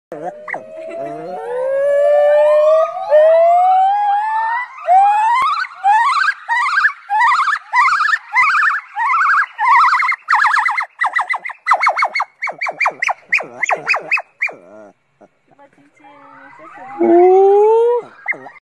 Couple of cool facts Gibbons are apes not monkeys
Video/Caption by @therealtarzann

A gibbon also as seen above can communicate with other members of his troop to warn of nearby predators by “🗣Hoo’ing” also different variations of the loud has been detected and studied that a certain high pitch or low pitch can determine the type of predator 😮...now the same vocals are used to let nearby troop members know that food is nearby or found from foraging ! The same dialect can be used in seeing a significant other and mating rituals ! This is the “language” of a gibbon it sounds the same to us humans but to them it’s an entire vocabulary and context behind every single “Hoo” 📝
Hope you learned something today !